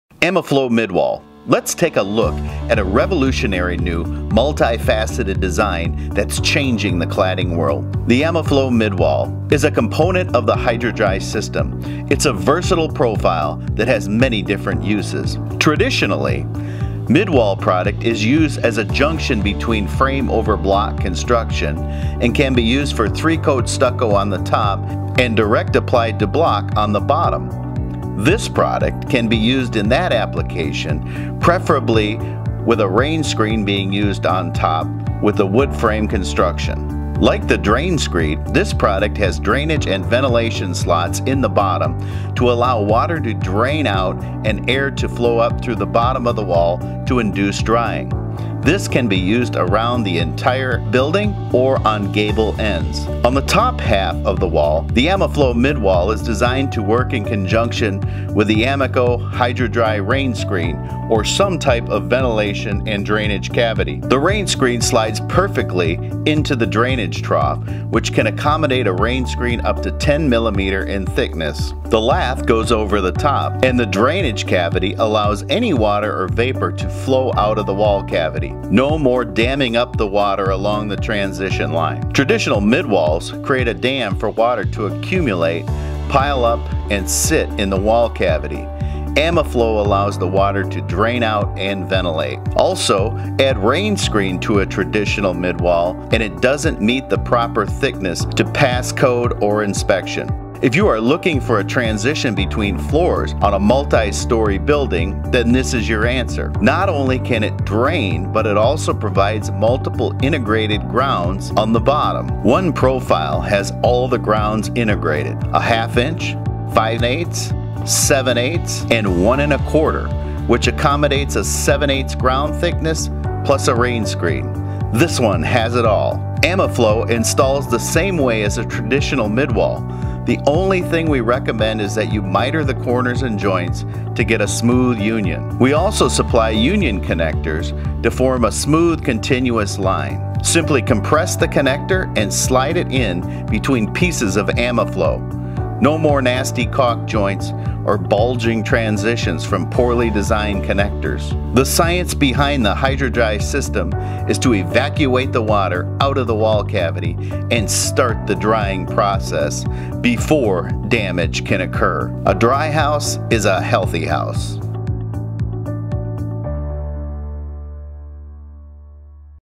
AMICO Amiflow Mid wall
The Amiflow midwall is a versitile component of the HydroDry self draining system.  It's applications span from use with multiple substrate materials like block and wood construction intersections to separating floors on mulit-floor light commercial projects.  The multiple ground thickness design make it suitable for almost any stucco or stone veneer application.